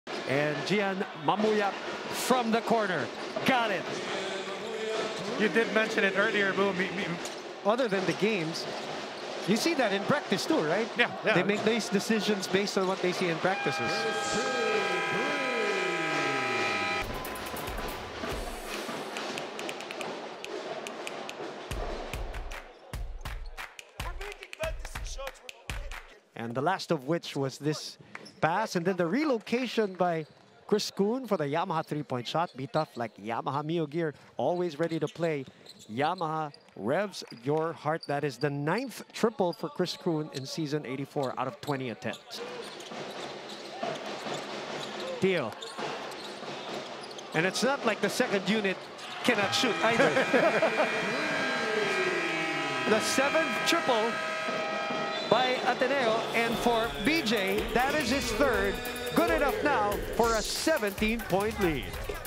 Ateneo second quarter run | UAAP Season 84 Men's Basketball
The Blue Eagles quickly turned the game around with three straight triples in the second frame against the Red Warriors!